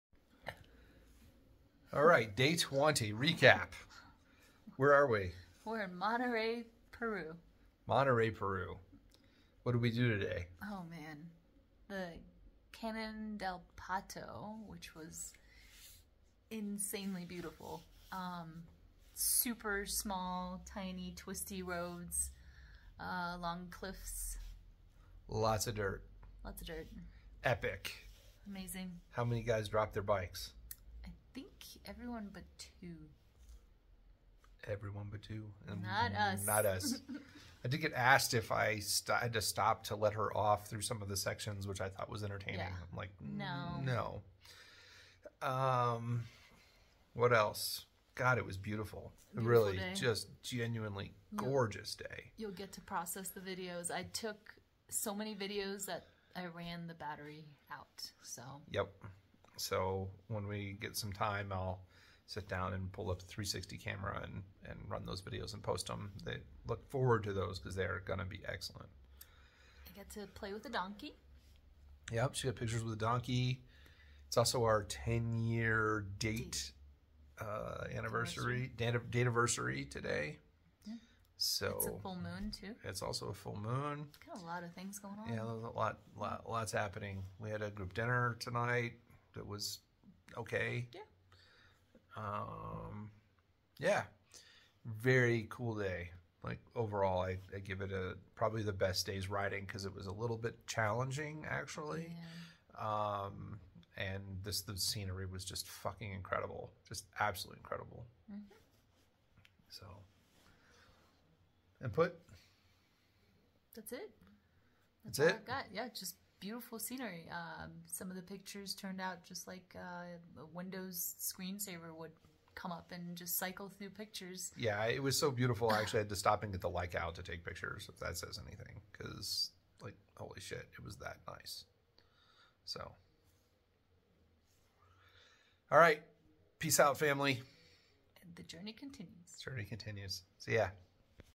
Peru! Day 20 Recap.
Today we did about 75 Miles of highway along the coast before making it to Canon Del Pato, by far the most beautiful and challenging riding of the trip.